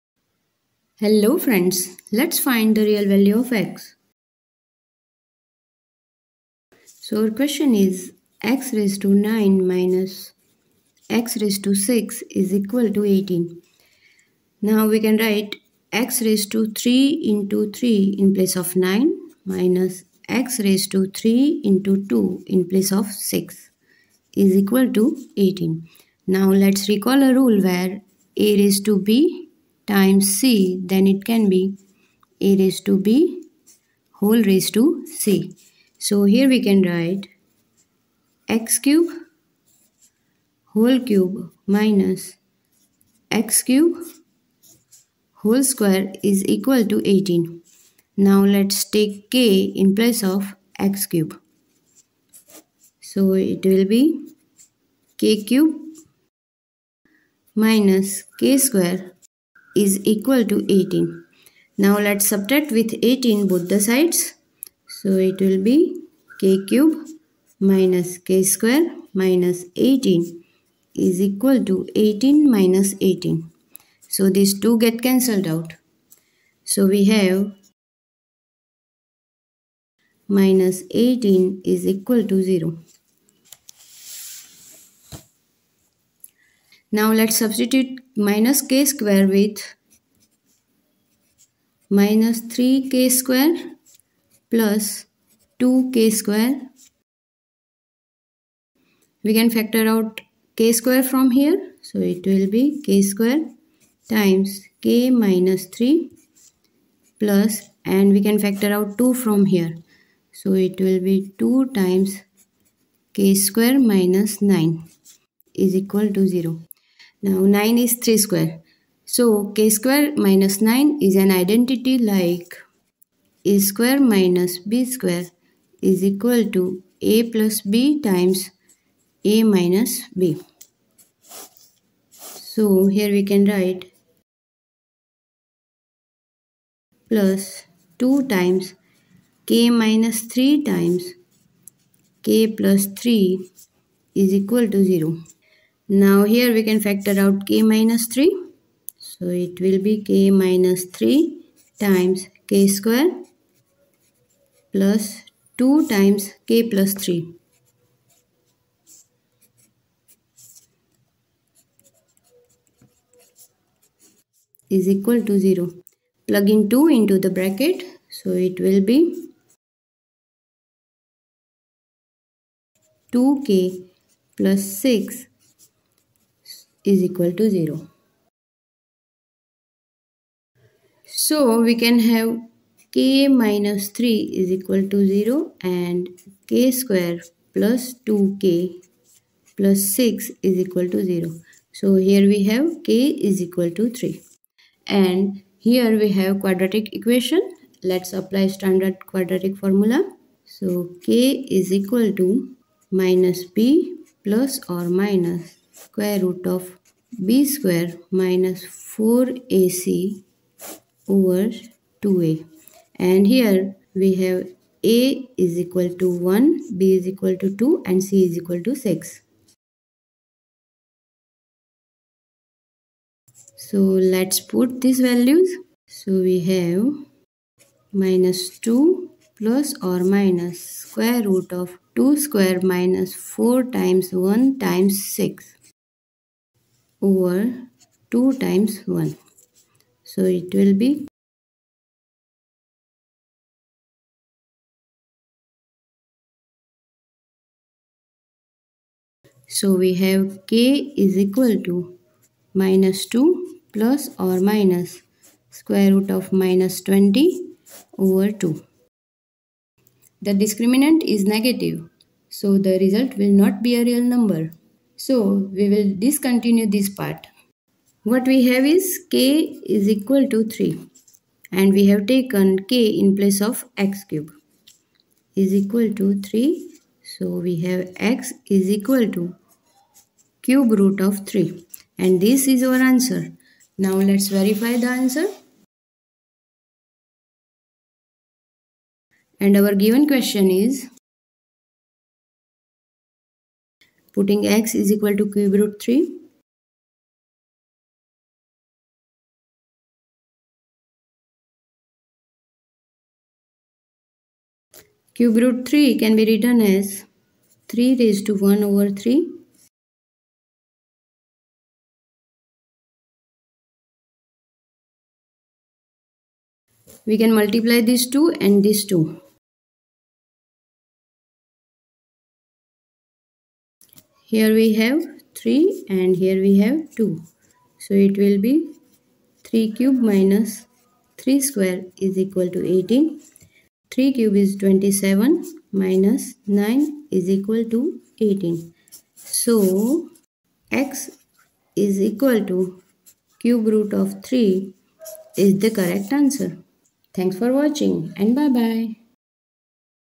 A Nice Algebraic Challenge | Do You Know This Trick? | x^9-x^6=18 Solve for the real value of x
x^9-x^6=18, Finding the real value of x.
Step by step, follow the video and pause if you want to try it for yourself.
Middle school math aptitude question.
Solve a cubic equation.